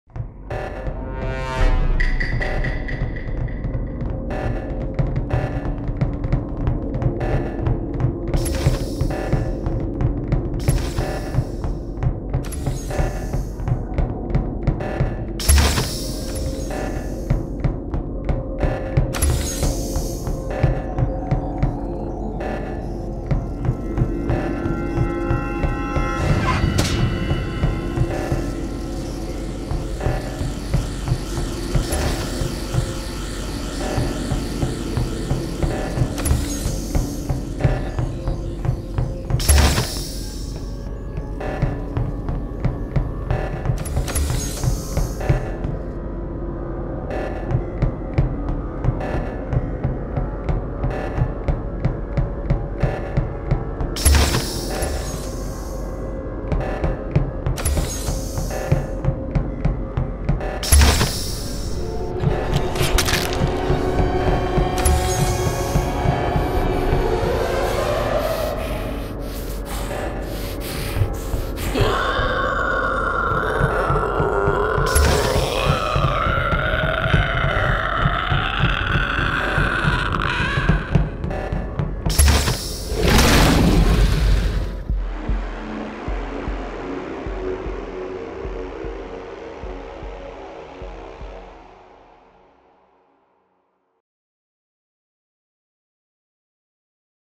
360 / VR Among Us Alien Infested Haunted Ship
Explore the Among us Ship, which has been infested by an alien presence, in 360 vr, from the comfort of home.
For the best experience, watch this video with your favorite virtual reality viewer.

Among us models were made by PLUSHii